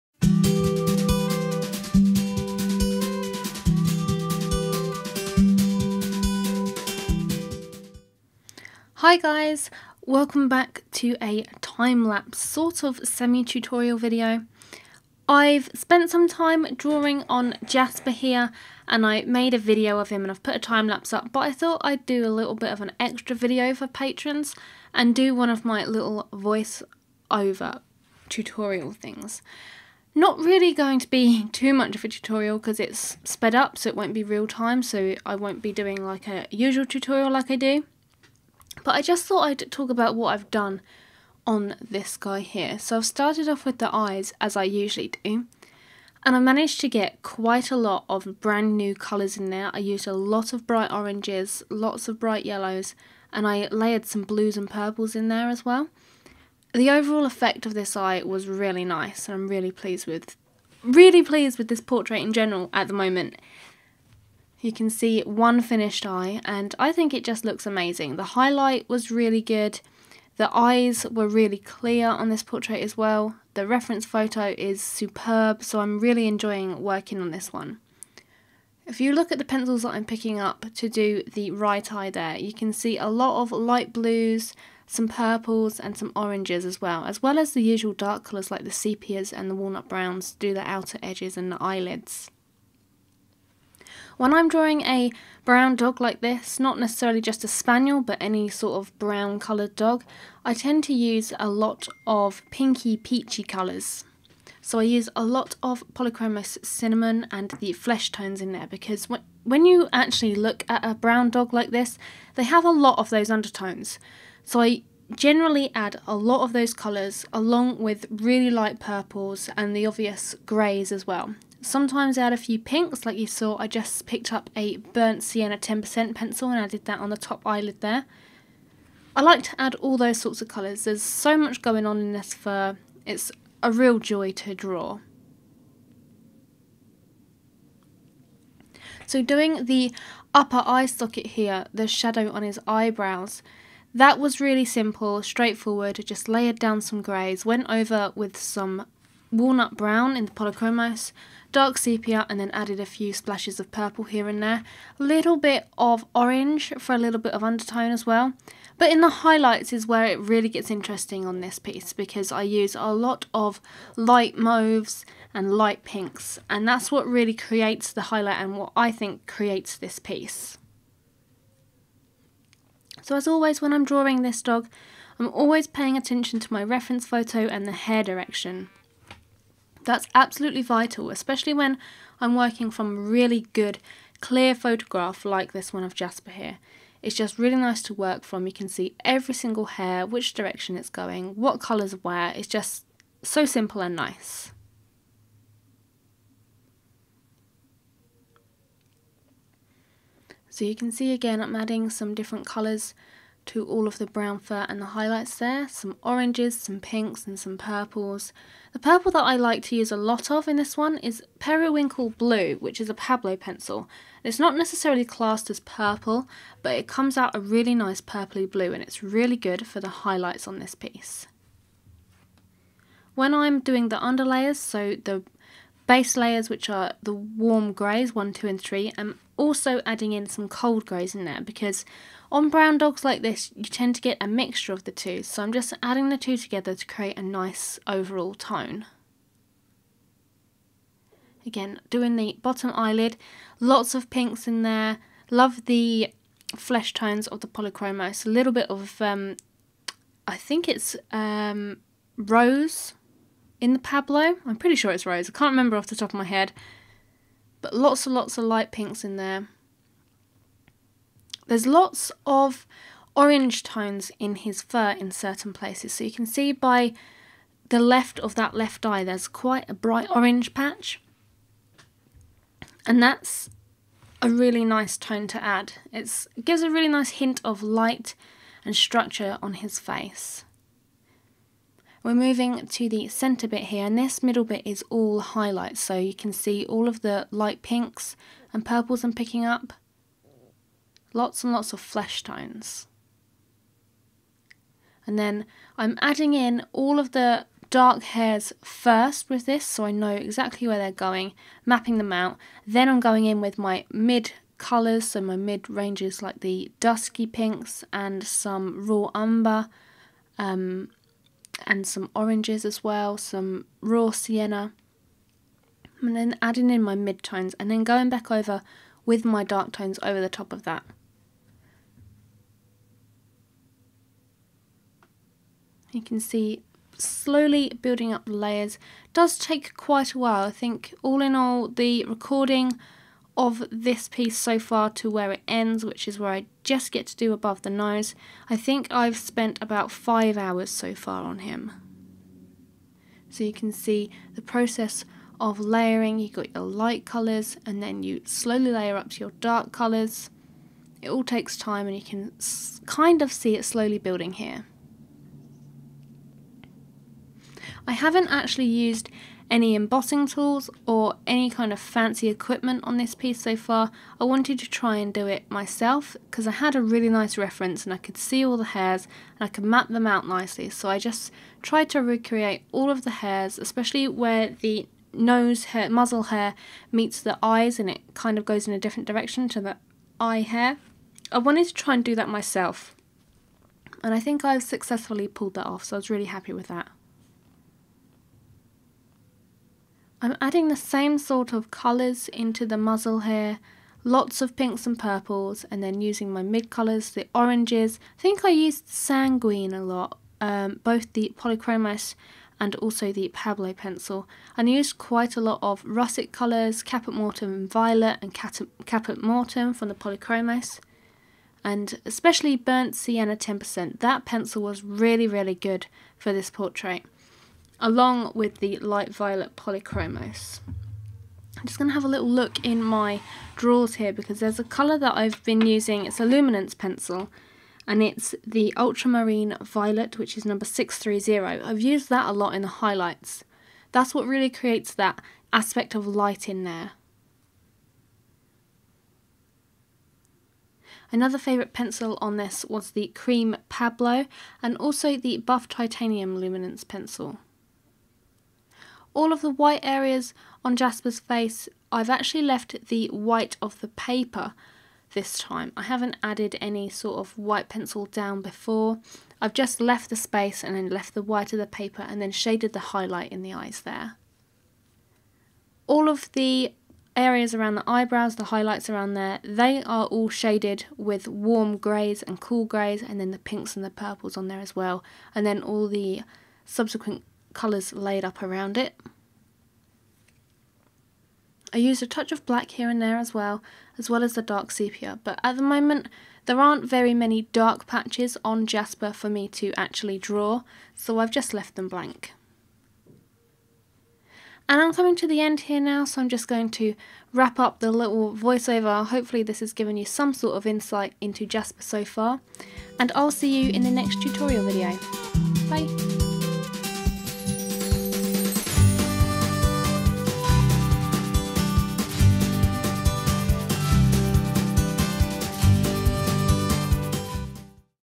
Jasper the Spaniel - Voiced Time Lapse Tutorial
This little voiced time lapse was originally for Patreon subscribers only, but I have decided to make this public so YouTube viewers can once again have a little peak at what I offer over there :)

You NEED to watch this! "BUSINESS SKILLS FOR ARTISTS | What you need to succeed "